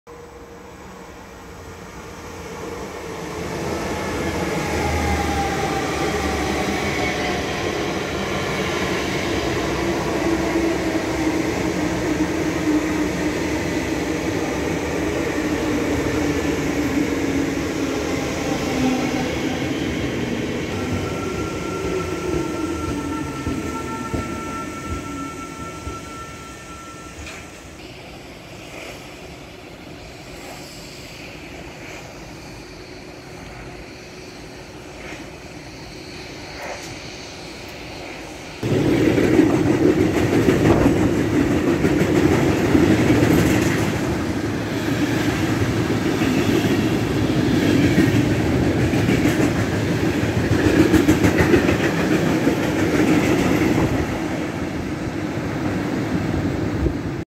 NS | SGMm 2995 met afscheidstekst te Zwijndrecht richting Dordrecht!
De laatste dagen van de SGMm stellen zijn begonnen. Ook NS besteedt er aandacht aan!

/ Sprinter / SGM / Stadsgewestelijk / Materieel / Treinstel / Oldskool / Oude / Buitendienst / Einde /